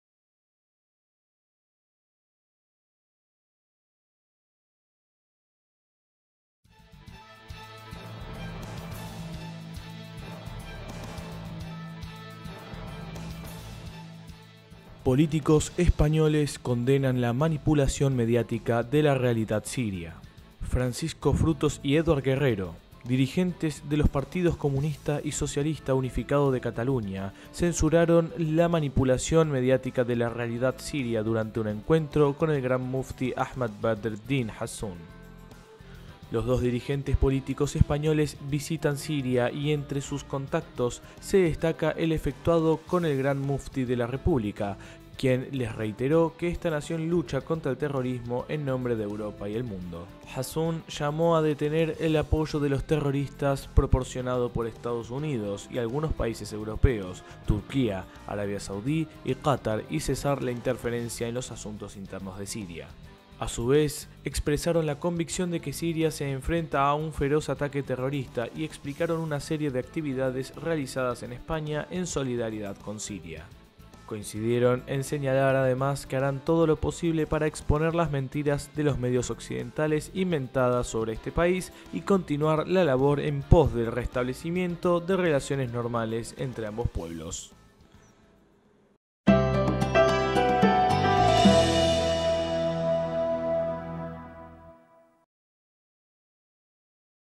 Políticos españoles condenan la manipulación mediática de la realidad siria
Francisco Frutos y Edward Guerrero, dirigentes de los partidos Comunista y Socialista Unificado de Cataluña, censuraron la manipulación mediática de la realidad siria durante un encuentro con el gran Mufti Ahmad Badreddin Hassoun.